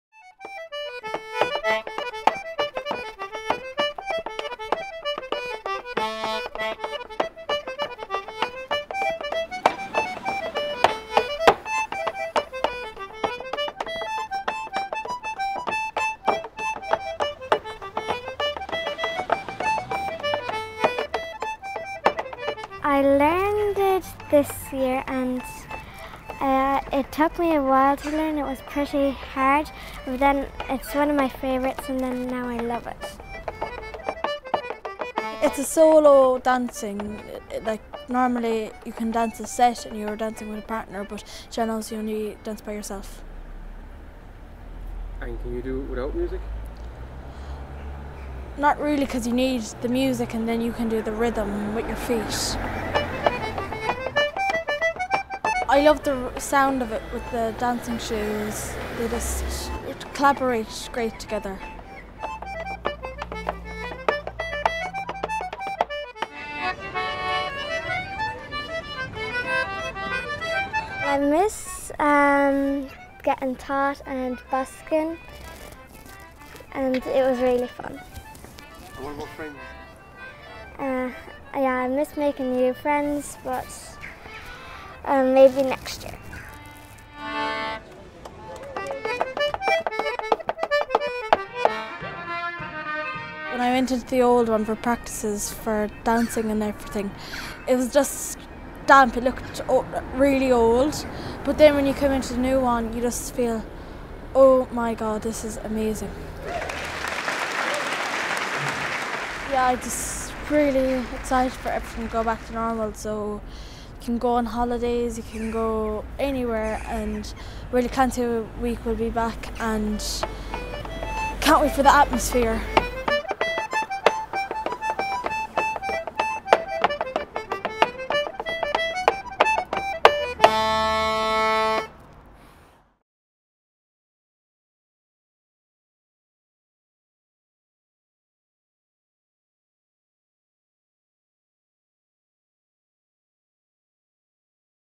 Taidgh and Raighnaid Griffin
Taidgh and Rainighd Griffin perform live on the streets of Miltowny Malbay, Co. Clare. Produced by Neil Hynes, Miltown Malbay Community Centre.